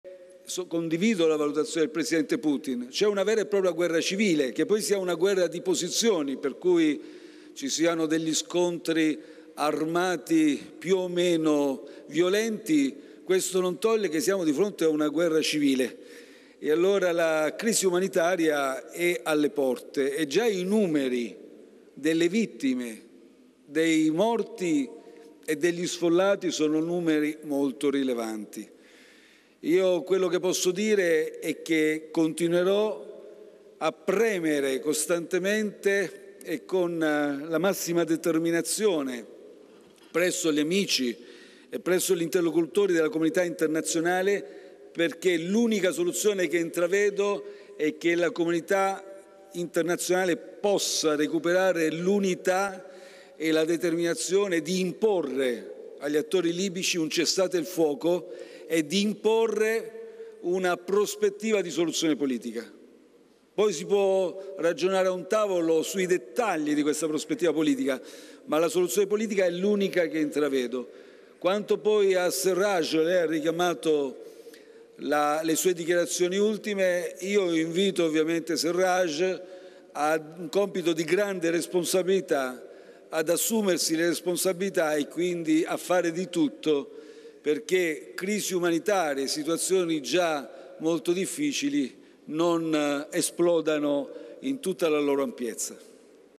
Conte: "In Libia c'è la guerra civile"
(Agenzia Vista) - Roma, 4 Luglio 2019 - La visita del Presidente della Confederazione Russa Vladimir Putin a Palazzo Chigi, con l'incontro con il Presidente del Consiglio Giuseppe Conte. Al termine del vertice i due leader hanno tenuto una conferenza stampa nel cortile del palazzo.